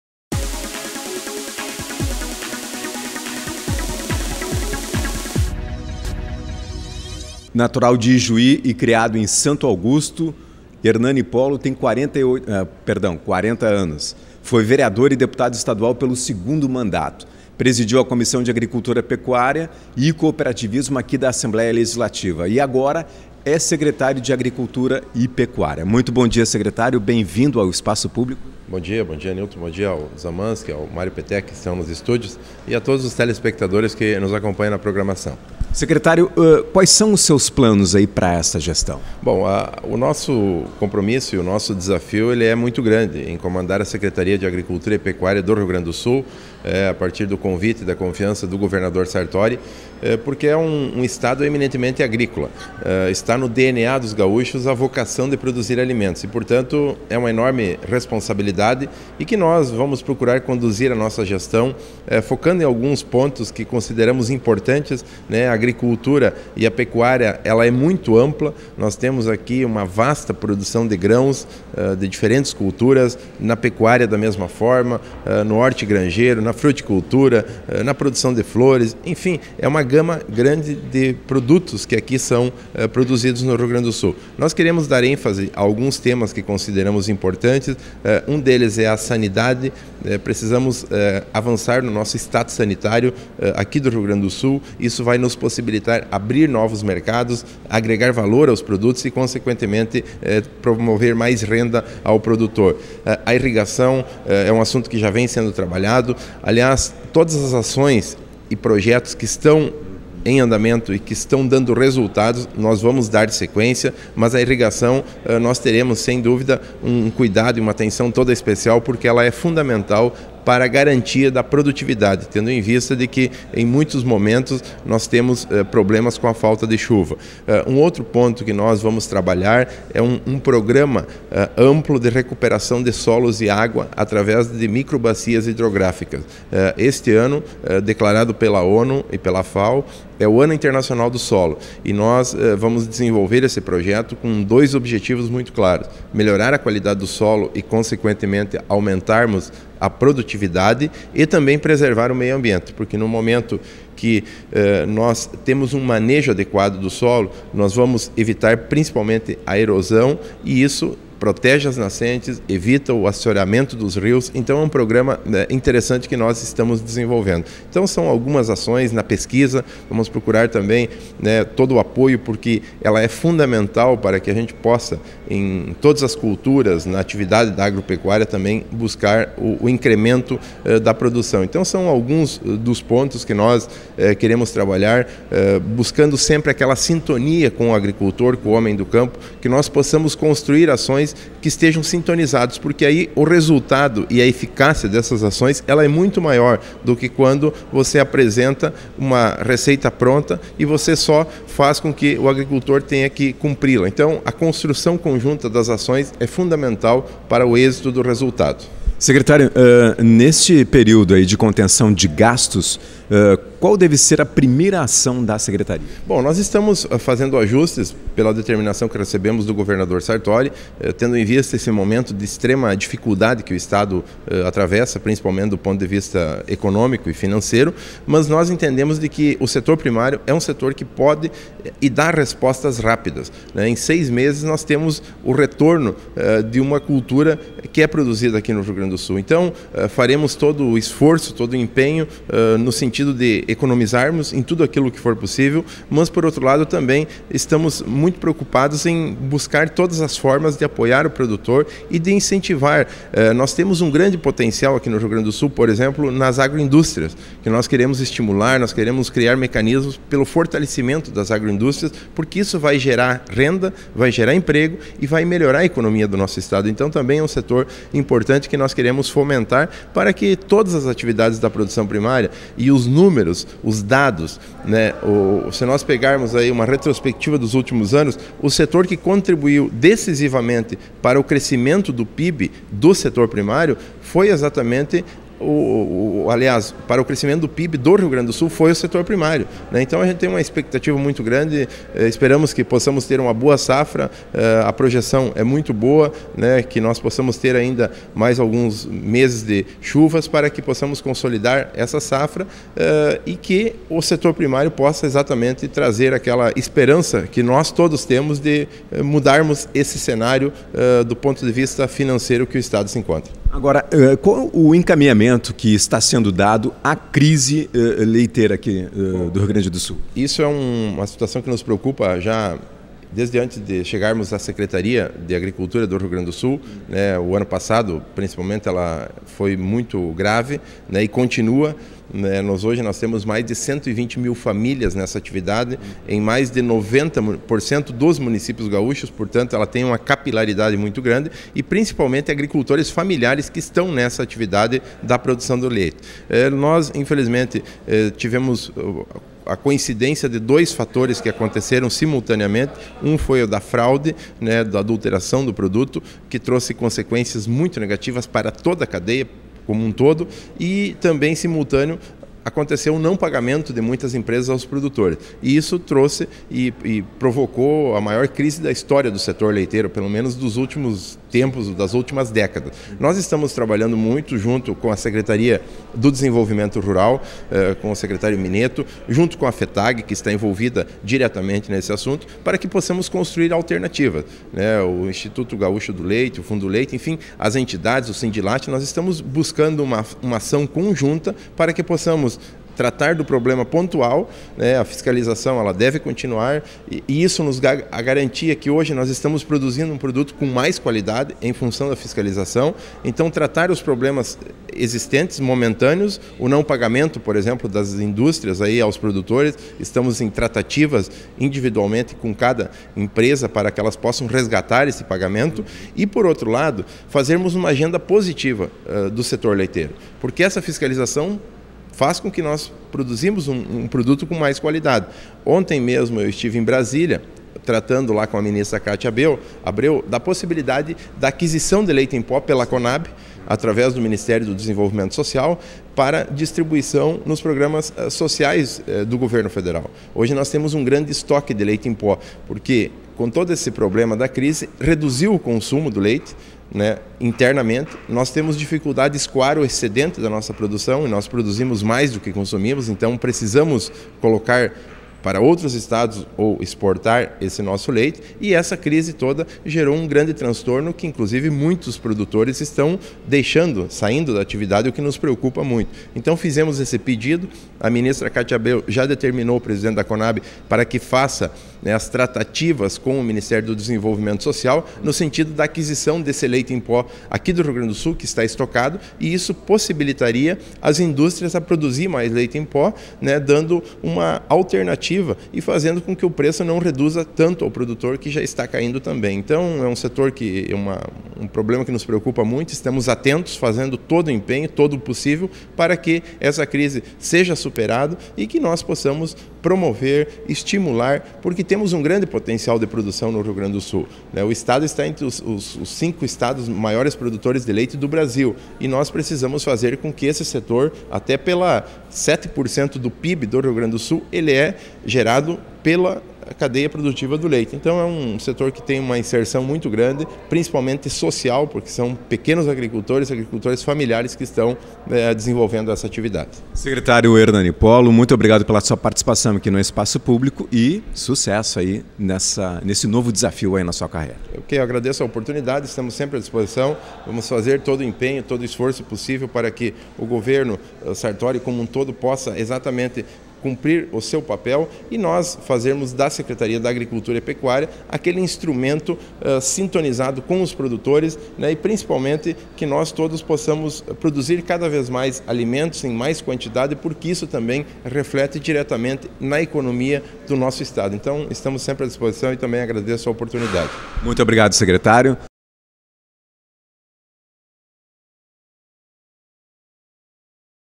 Secretário Ernani Polo fala sobre planos para a Agricultura
Secretário da Agricultura e Pecuária, Ernani Polo, fala sobre planos para sua gestão.